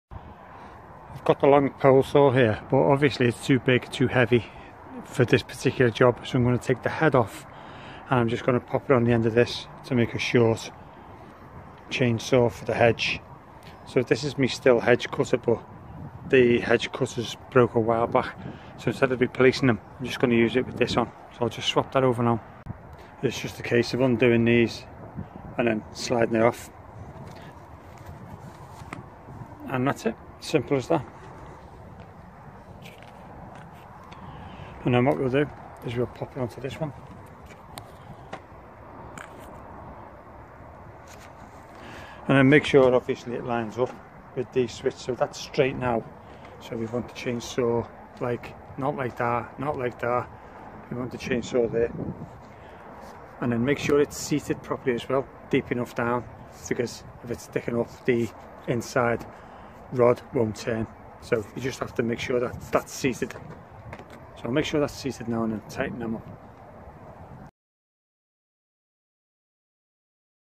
Adapting stihl hedge cutter into handy short reach chainsaw
For this adaption im lucky enough to have a pole pruner to borrow the chainsaw from but you could always buy a chainsaw head. It is a great bit of kit as it is not too heavy. Hope this comes in useful for someone.